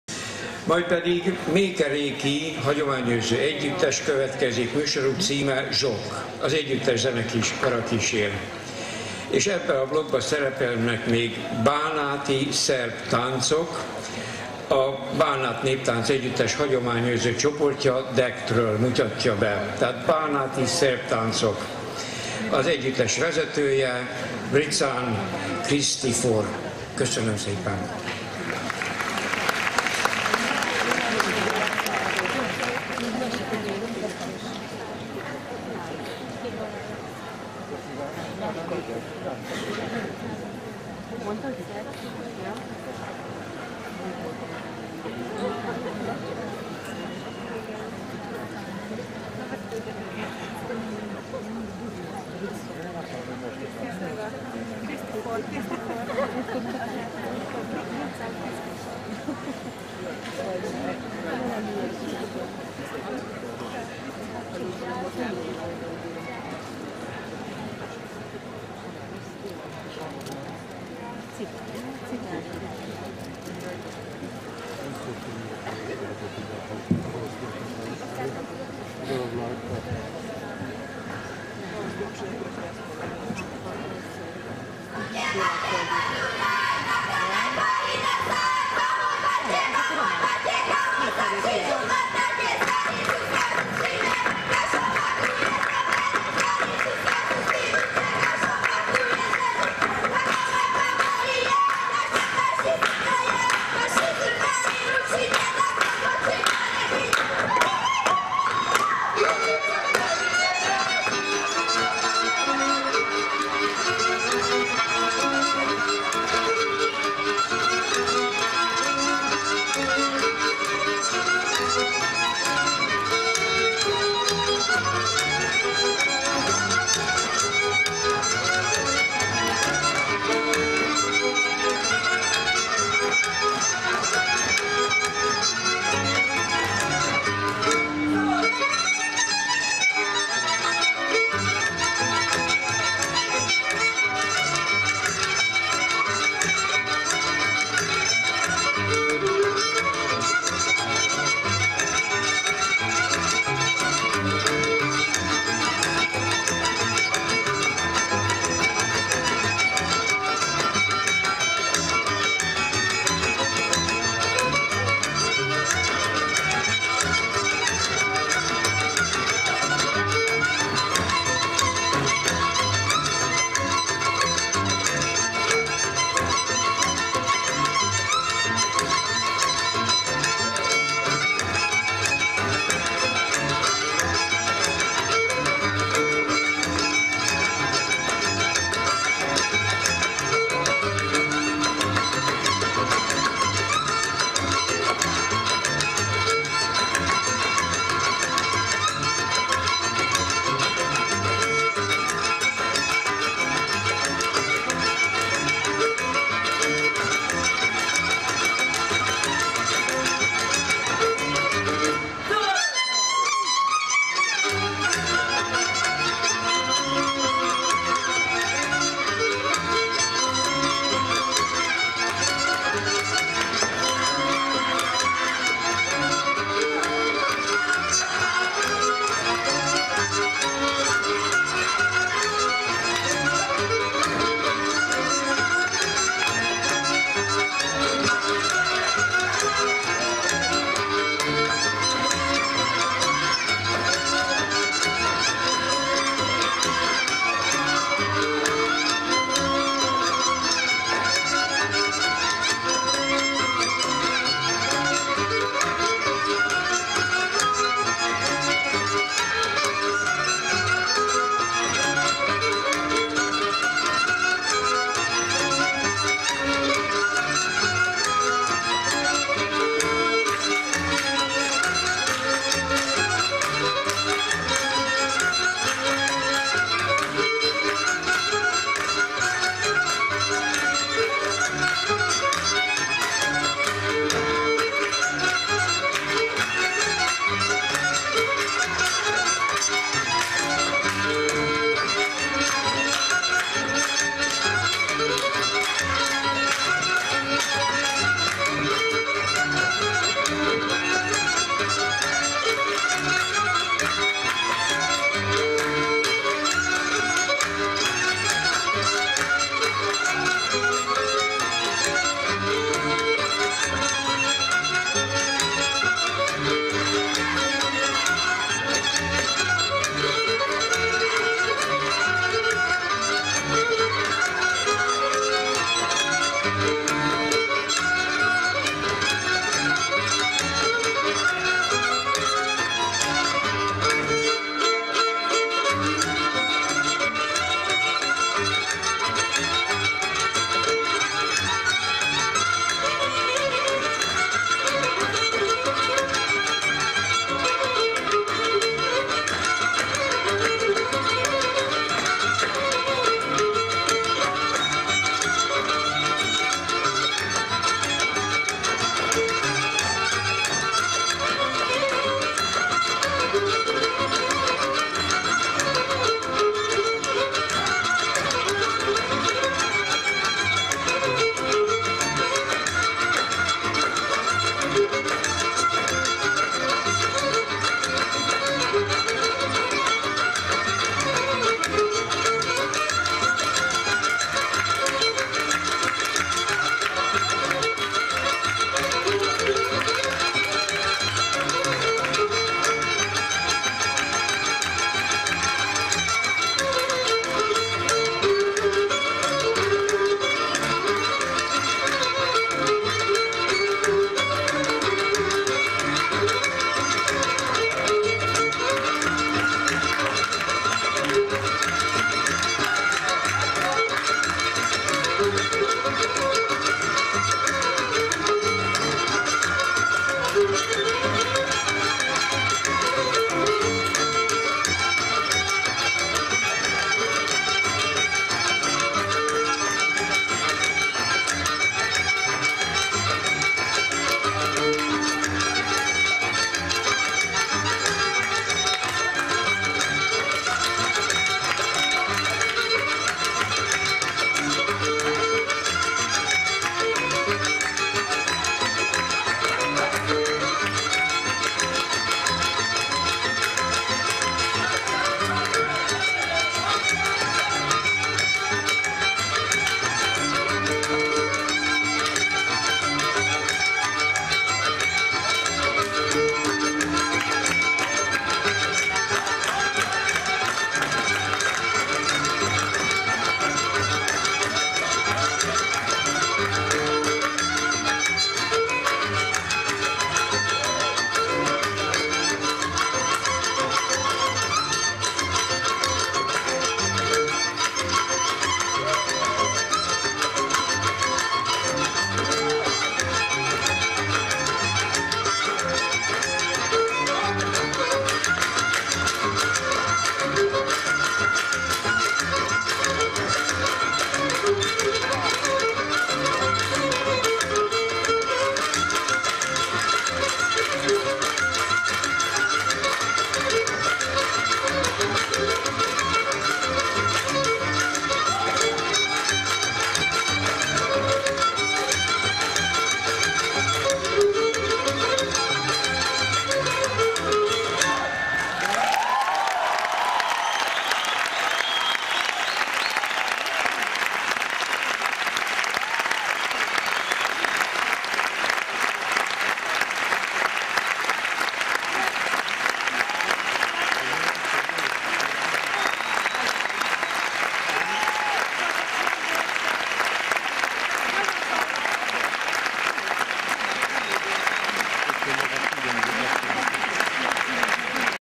Méhkeréki táncok (Román táncok) - 2004 Táncháztalálkozó - V - ルーマニア民族舞踊
Méhkeréki zenekar
XXIII. Táncháztalálkozó és Kirakodóvásár   2004. március  28. Budapest
SYMA Rendezvénycsarnok és Körcsarnok